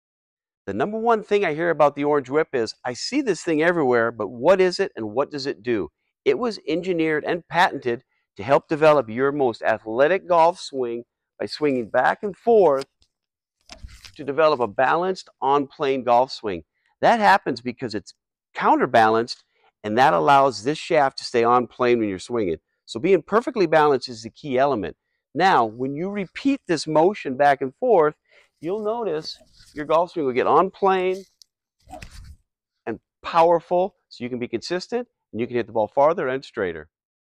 What Does The Orange Whip Trainer Do?
The Orange Whip was engineered and patented to develop an athletic golf swing to stay on plane, balanced, and powerful. The patented counterbalance keeps your golf swing on plane so you can be more consistent on the golf course.

FOLLOW Orange Whip Golf & Fitness on social media @orangewhipgolf